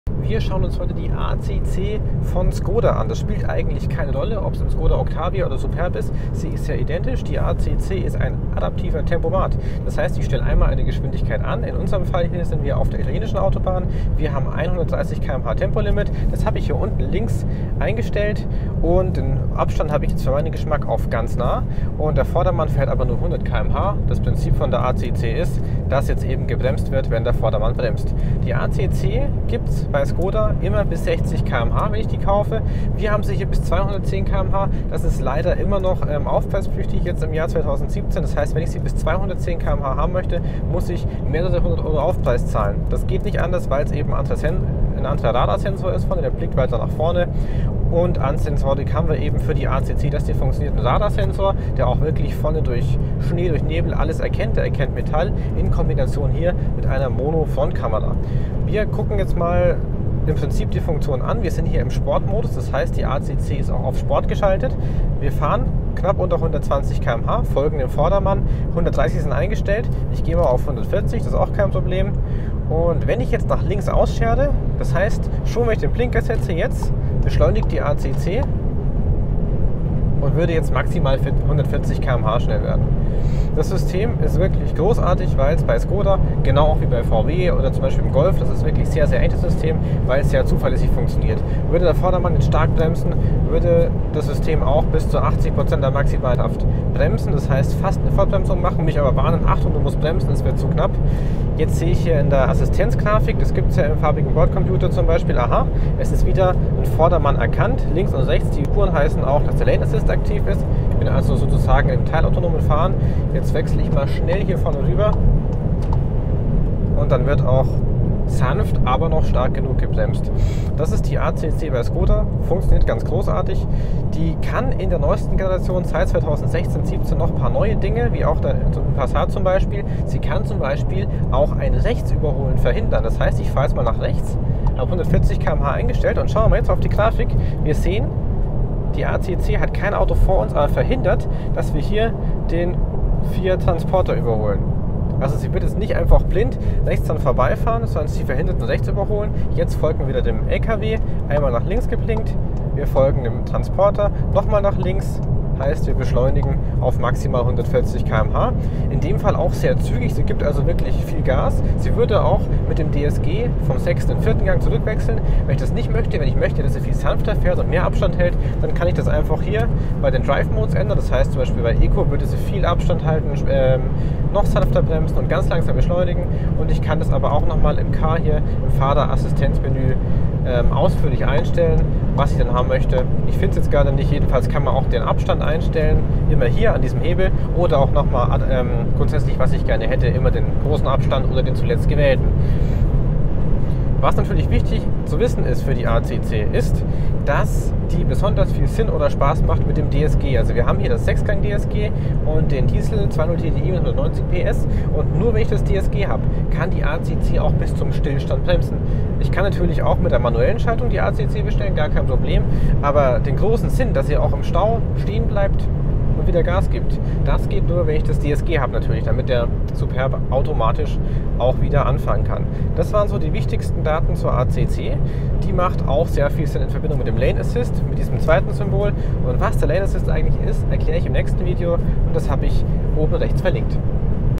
ACC bei Skoda/VW/Audi/Seat: Erklärung der adaptiven Geschwindigkeitsregelung
Die adaptive Geschwindigkeitsregelung (ACC, adaptive cruise control) ist sozusagen eine Erweiterung des klassischen Tempomats: Sie hält eine eingestellte Geschwindigkeit konstant, kann allerdings (wenn man dem vorausfahrendem Fahrzeug zu nahe kommt) auch abbremsen und (z.B. beim Überholen) wieder anfahren. Besonders viel Sinn macht eine ACC in Verbindung mit einem Automatikgetriebe, da in Stausituationen häufig schon bis zum Stillstand gebremst und vollautomatisch wieder angefahren werden kann.
Wir zeigen die ACC in einem Skoda Superb, sie funktioniert nahezu genau so aber im kompletten VW Konzern, also auch z.B. im VW Golf, dem Skoda Octavia, Seat Leon oder bei Audi.